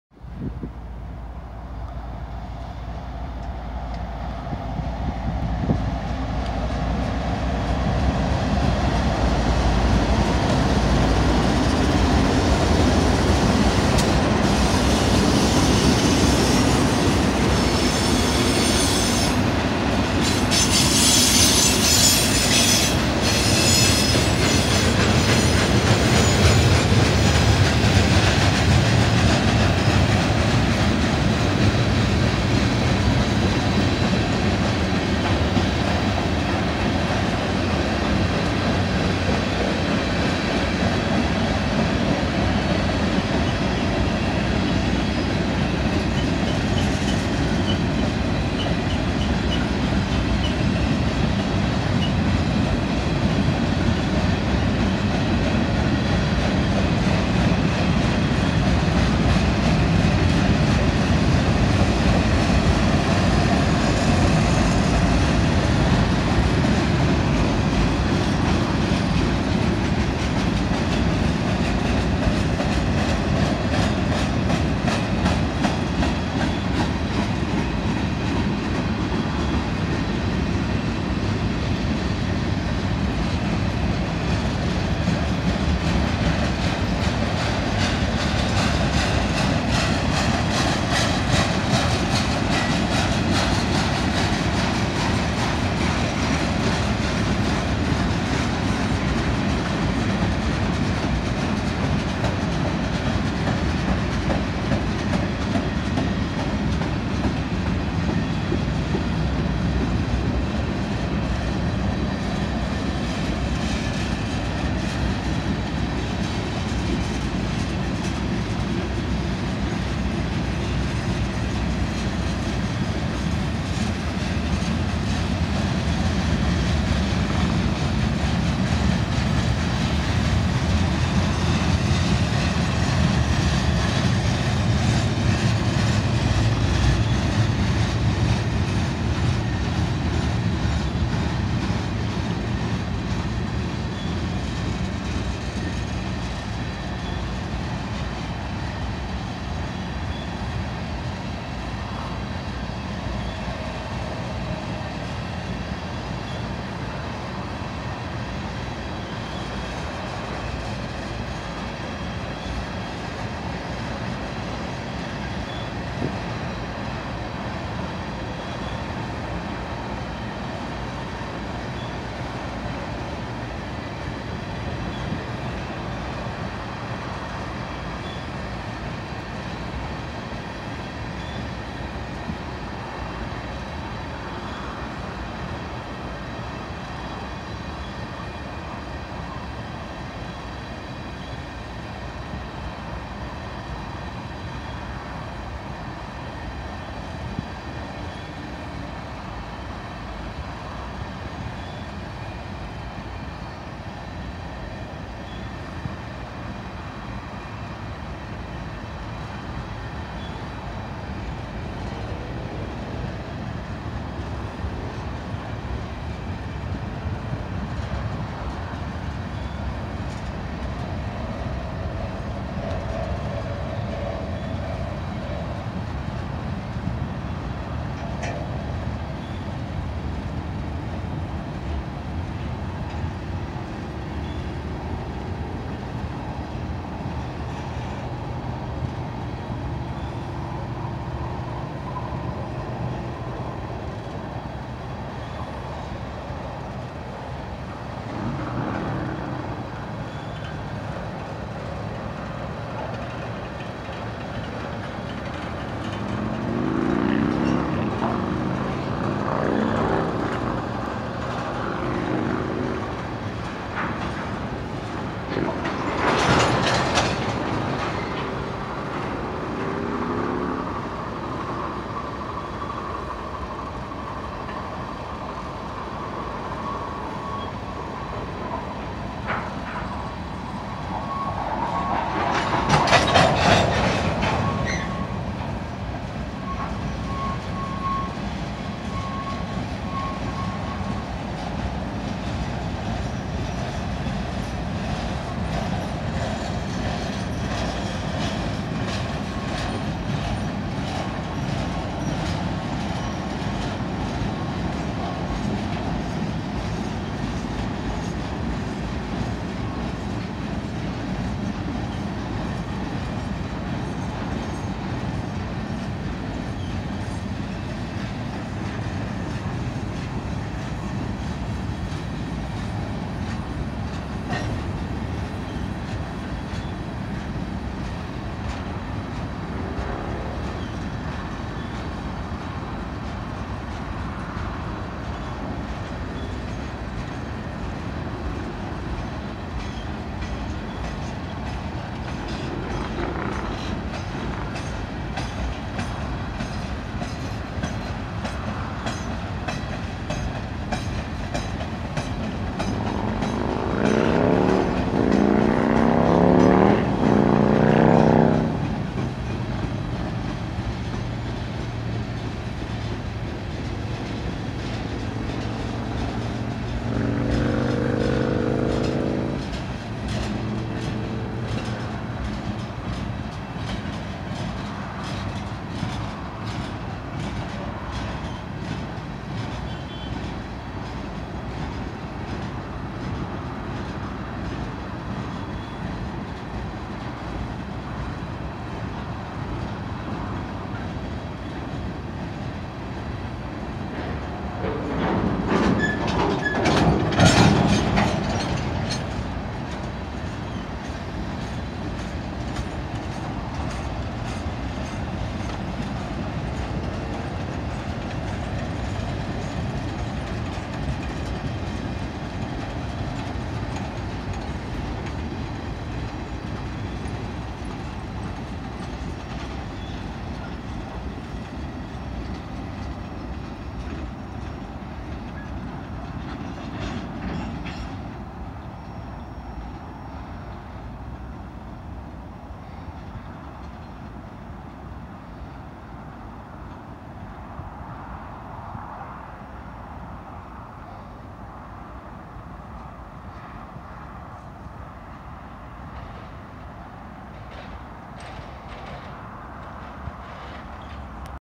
WB BNSF Manifest Train Feat Bluebonnet At Lenwood Ca!
A westbound BNSF manifest train feat bluebonnet trail fail went by here at Lenwood Ca!

7302-ES44DC (BNSF) (Leading Unit)
7577-ES44DC (BNSF) (Trailing)
4444-Dash-9C44-9W (BNSF) (Trailing)
5359-Dash-9C44-9W (BNSF) (Trailing)
142-GP60M-3 (BNSF) (Trailing)
189-GP60 (ATSF) (Bluebonnet) (Trailing)
151-GP60M-3 (BNSF) (Trailing)